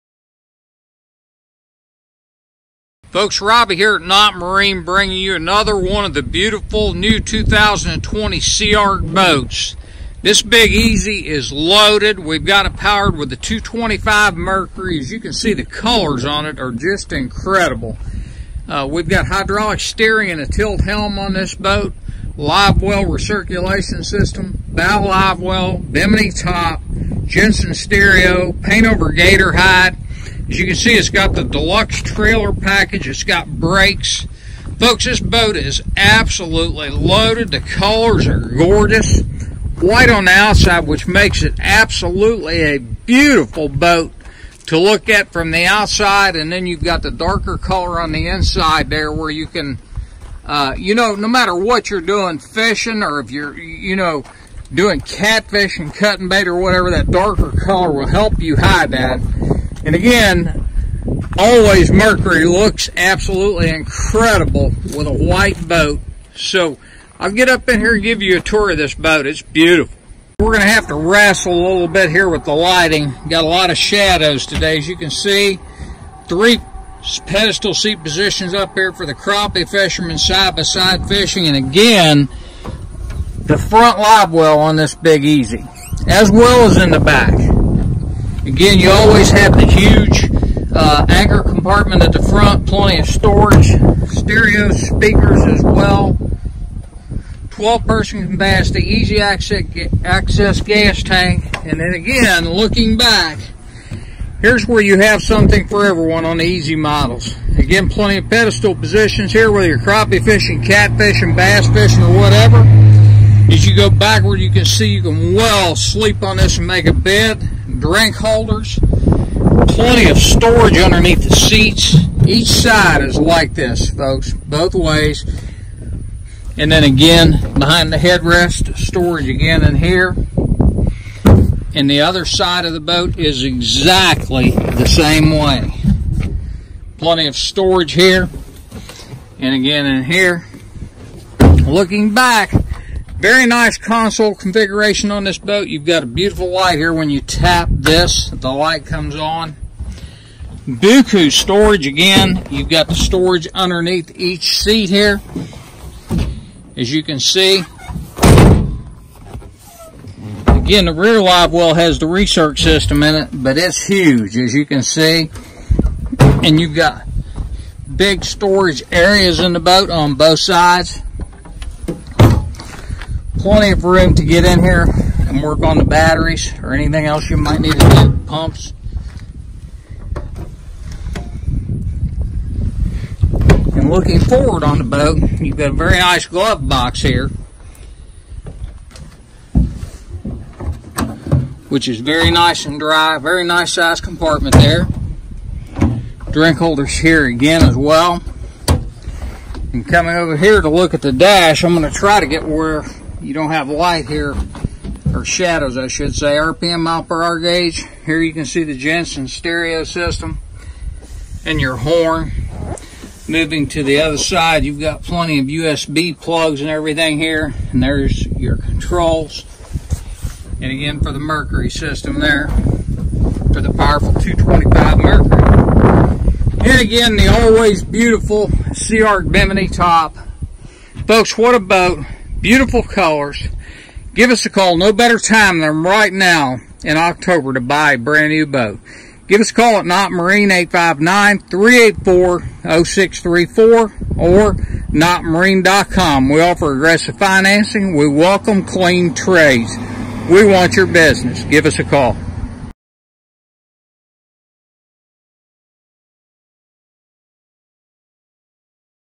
SeaArk Big Easy
Mercury 225, SeaStar Hydraulic Steering, Livewell Recirculation System, Round Radio Kit, Bimini Top, Mystic White Paint with Gatorhide.

Check us out on:
Facebook (Knot Marine)

We will be happy to help you with any of your boating needs.